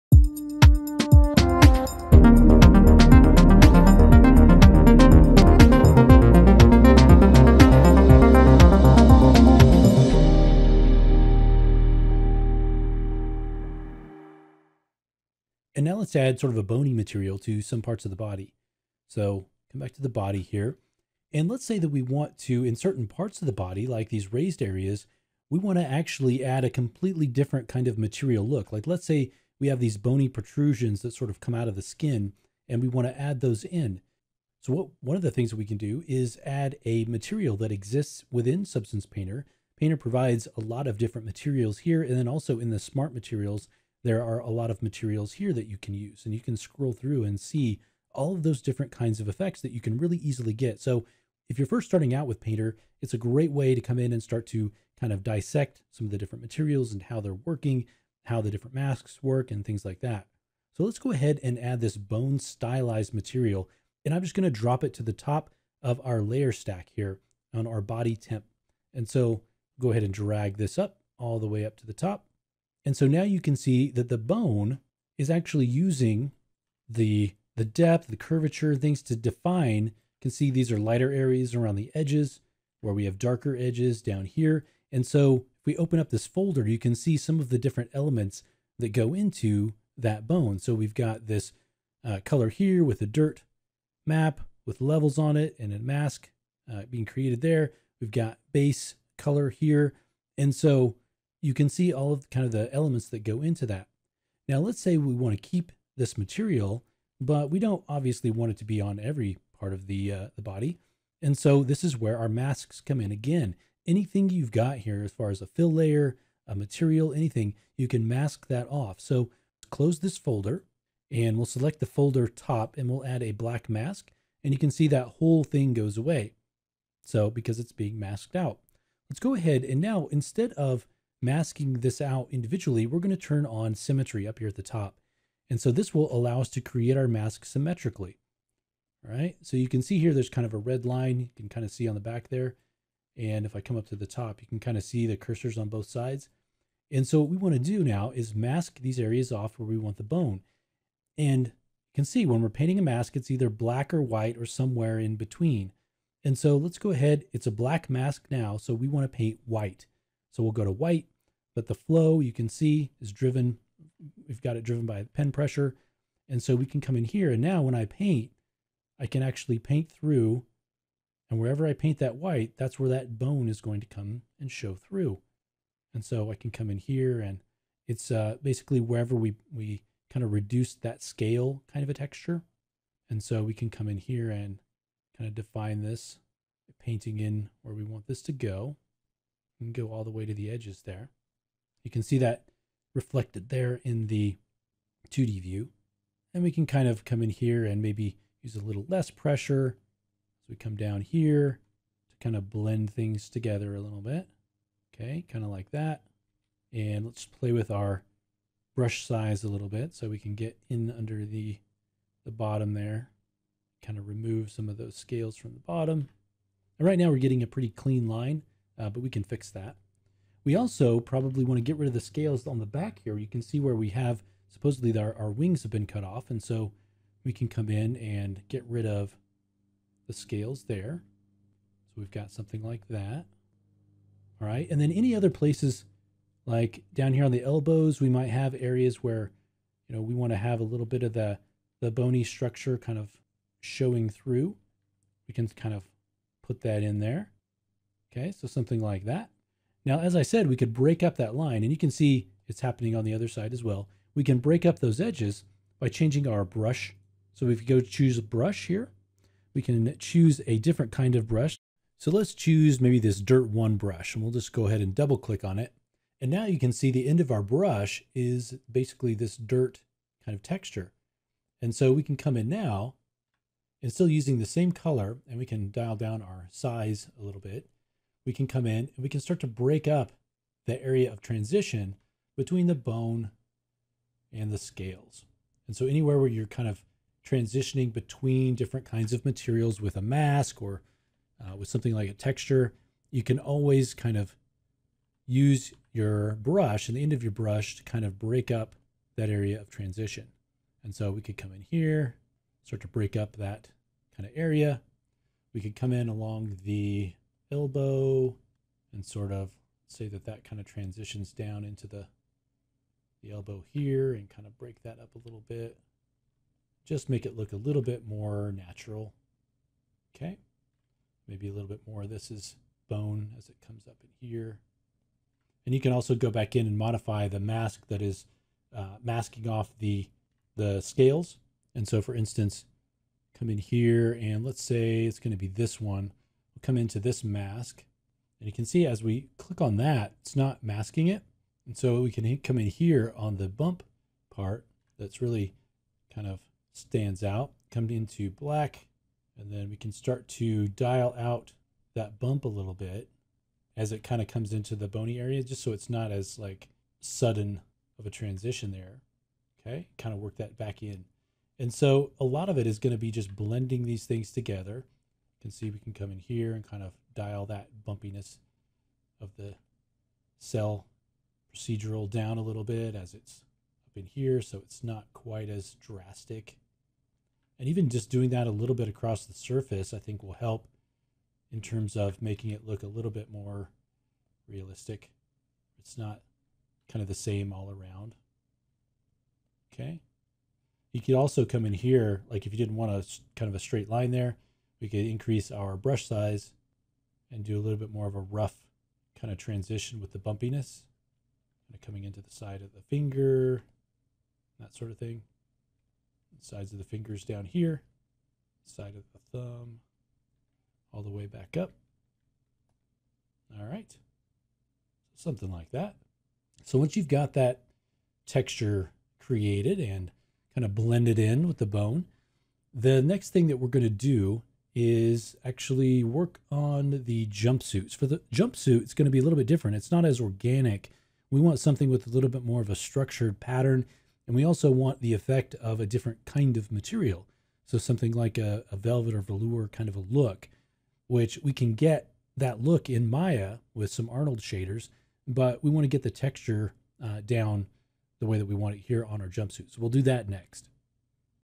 Creature Texturing for Film - Lesson 4 / 18
Combine multiple materials on a single mesh to create bony protrusions. Tutorial credit: Justin Marshall Check out the whole series